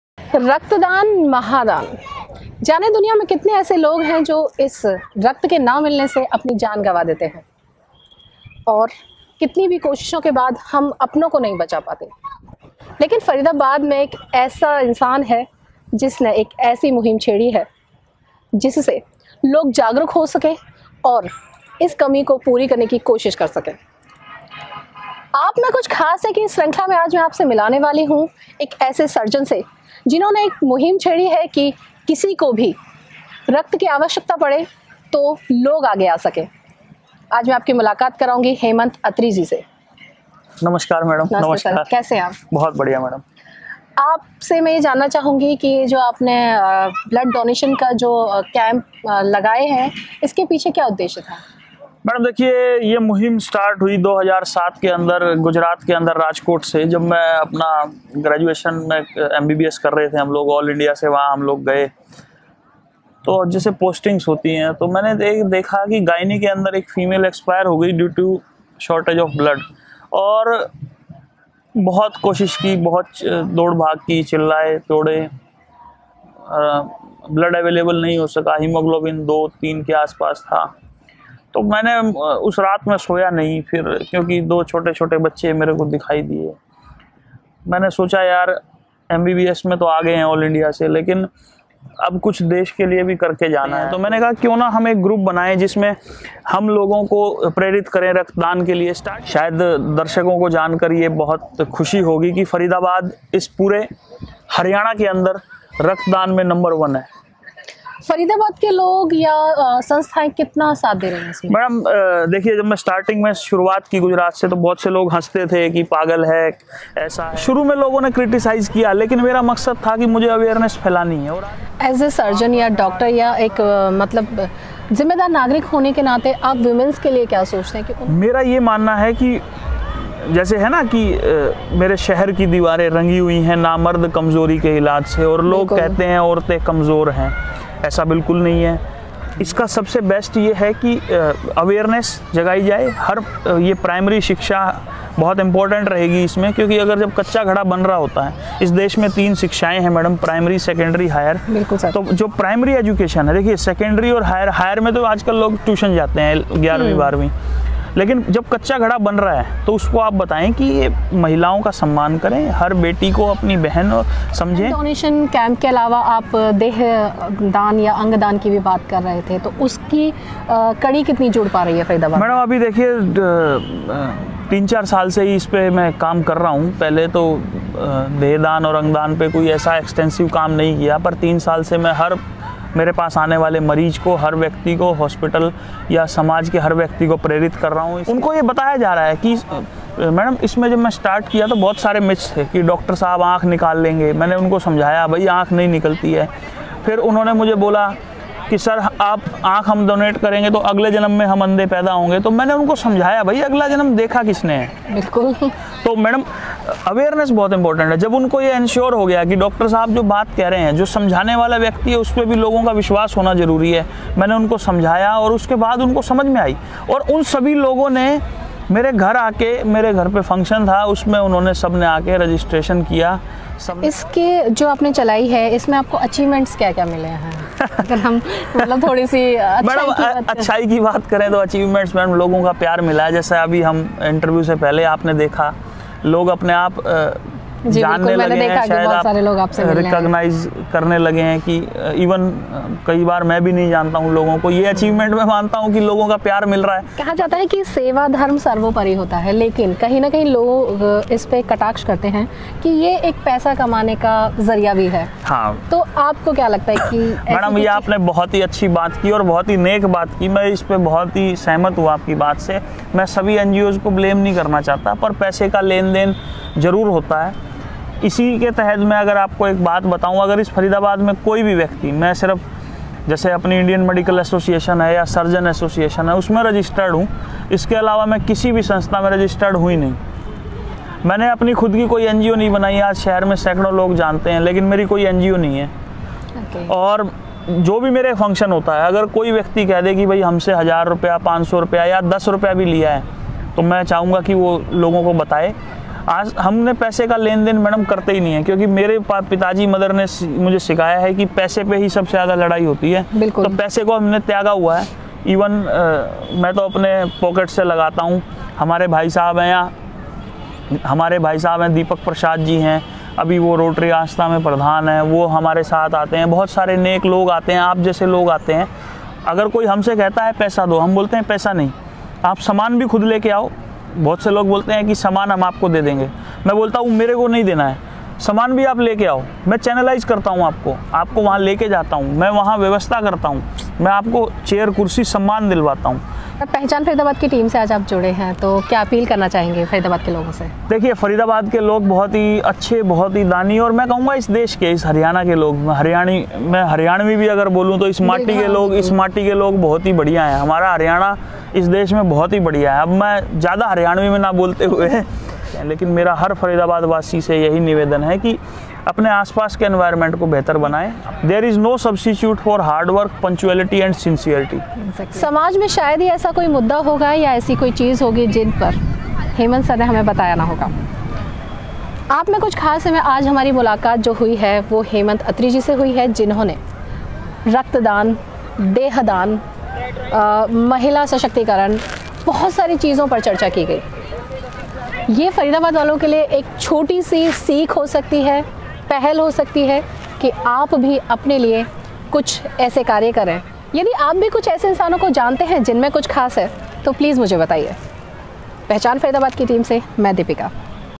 BLOOD-DONATION KING - Dr.Hemant Atri
100 से भी ज्यादा रक्तदान शिविर लगा चुके और लोगों को जागरूक करने वाले डॉ हेमंत अत्री ने एक ऐसी मुहिम छेड़ी है जिससे समाज को एक नई सीख मिल पाएगी  रक्तदान हो या अंगदान दोनों ही समाज के लिए कितनी जरूरी है आप में कुछ खास है कि इस श्रंखला में आज की मुलाकात है डॉ हेमंत अत्री से